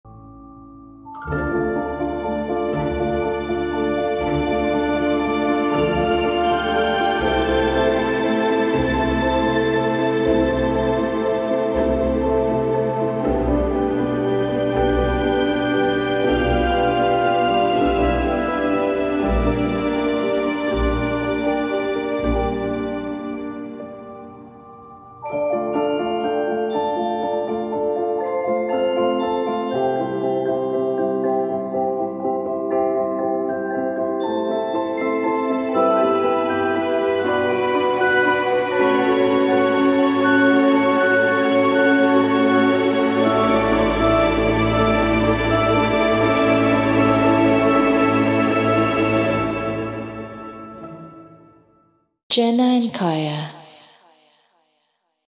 Jenna&Kaia Prive presents Mettasha.
Mettasha Collection believes in the power of hope and a brighter future, even in challenging times. It symbolizes embracing new hopes for humanity.

This collection combines comfort and style. Soft materials provide a pleasant feel, enveloping the wearer in soothing comfort. Layering adds depth and versatility to the designs, while intricate embroidery showcases meticulous craftsmanship.

This collection encourages embracing hope and renewal. It reminds us that even in challenging times, optimism and a brighter future are possible.